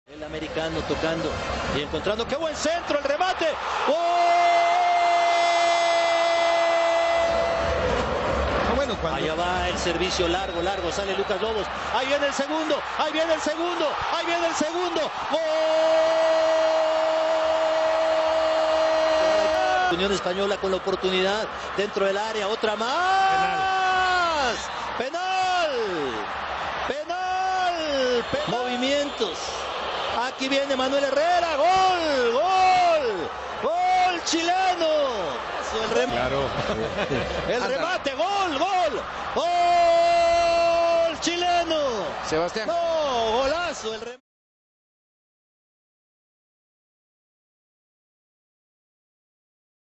Deja ir Tigres la Libertadores
Tigres, que juega con equipo B, deja ir ventaja de 2 goles y empata 2-2 con Unión Española, para quedar fuera de la Libertadores con global de 3-2.